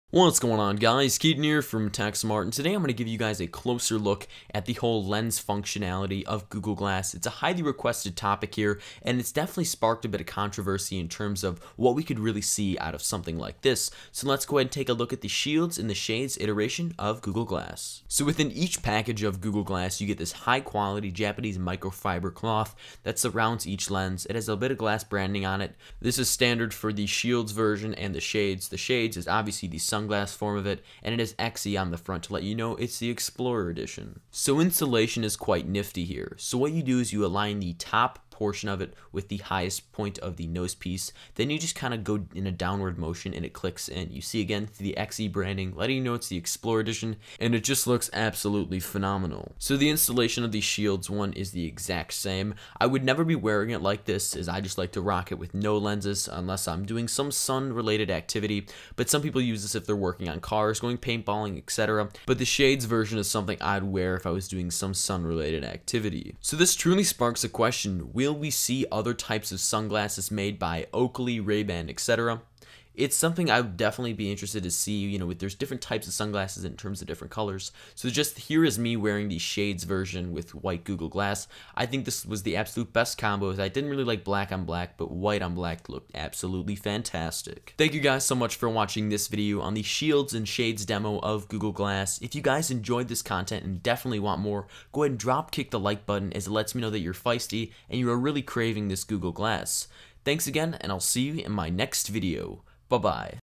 Google Glass Lenses Hands-on
Thumbs up for Google Glass's interchangeable lenses!

Google Glass as a unit works well. The technology within is superb, and is definitely the future of tech. One of the main functionalities within Google Glass is the interchangeable lenses. In the box, you receive the Shields and Shades. The Shields features a clear lens similar to a pair of safety-goggles, or eye-glasses. The Shades feature a tinted-lens that acts as sunglasses. Both of the lenses cater to a certain market at this moment in time. Now, a rumor has been circling stating that Google is alleged to feature a prescription-lens for users who'd like to optimize Glass's abilities, whilst being able to use them as standard glasses. The Shields have been left in their box due to the oddness of them. I typically rock Google Glass lensless, which is what most do. The Shades are a different story. They offer a shield between the death-defying sun, and your eyeballs. They look sleek, and are definitely something I use when going outside. In the end, this is the future for glasses technology, and the shades compliment the use of tech on yo' face!

What do you think of Google Glass's lenses? Do you think Google will partner with Ray Ban, or Oakley to truly optimize the user?